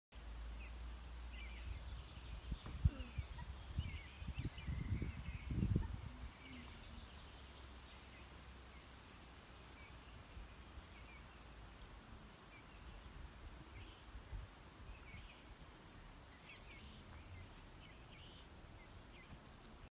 20 Sekunden Uckermark - Heimweg
Ein schöner Sommertag - Badetag neigt sich dem Ende. Gut hingehört und los gehts Richtung Heimweg...